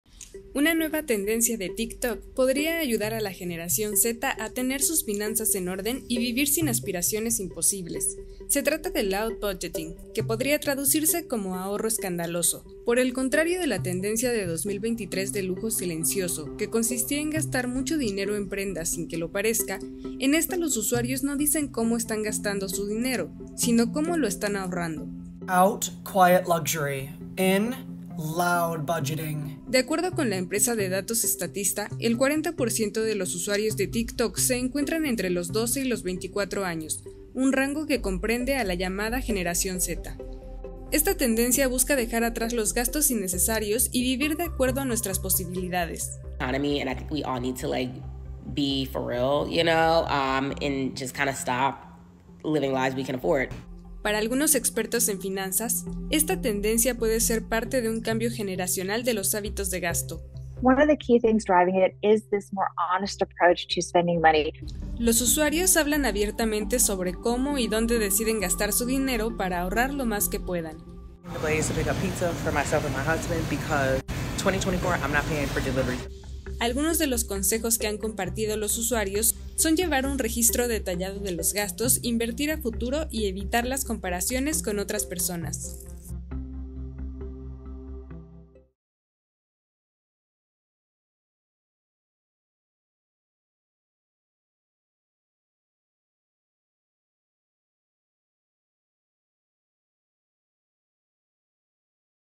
Conoce la nueva tendencia de TikTok que enseña a la Generación Z a hacer presupuestos realistas
El ‘loud budgeting’ es la nueva tendencia de TikTok sobre educación financiera que se está popularizando entre los jóvenes de Estados Unidos. Busca que los usuarios puedan ser más abiertos y realistas con sus presupuestos. Sigue las últimas noticias en Univision.

Maity Interiano